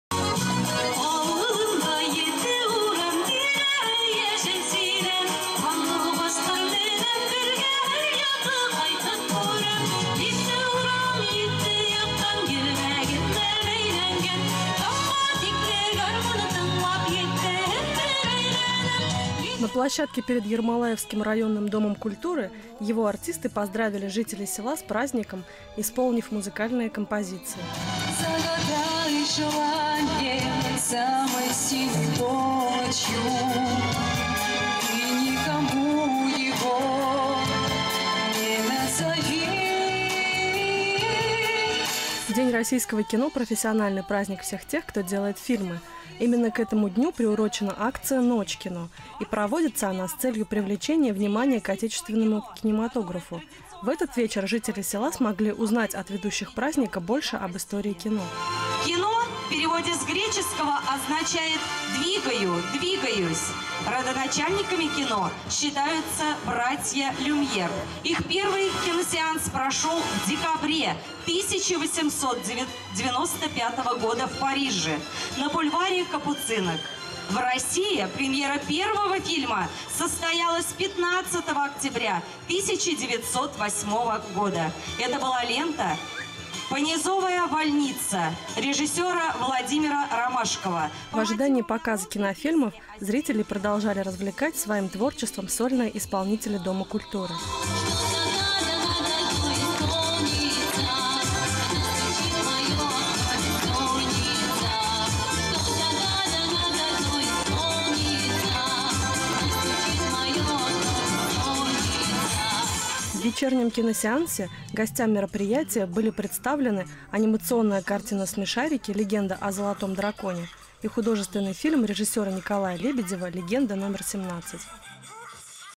"Ночь кино" в с. Ермолаево
В Год российского кино, традиционно отмечающийся с 1980 года, по всей стране проходит акция «Ночь кино». Впервые такая акция прошла и в селе Ермолаево.